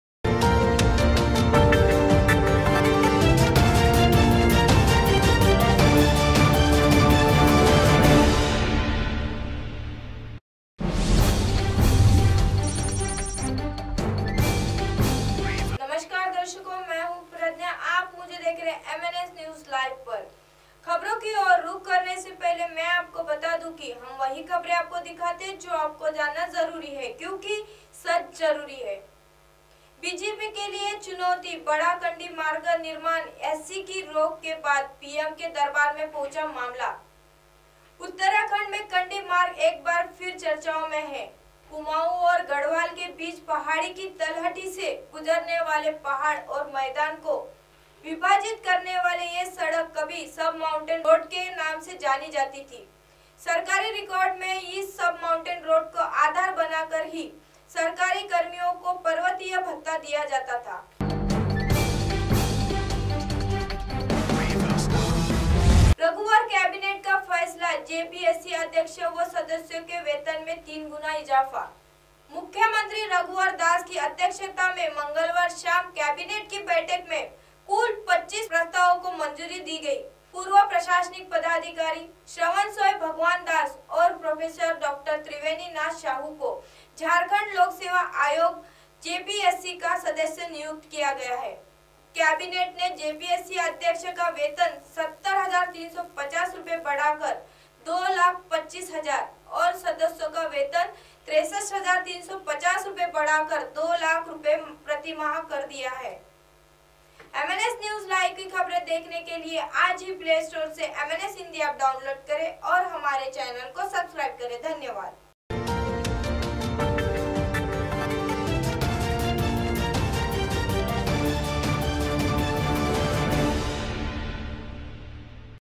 के लिए चुनौती बना कंडी मार्ग का निर्माण, SC की रोक के बाद पीएम के दरबार में पहुंचा मामला
BJP के लिए चुनौती बना कंडी मार्ग का निर्माण, SC की रोक के बाद पीएम के दरबार में पहुंचा मामला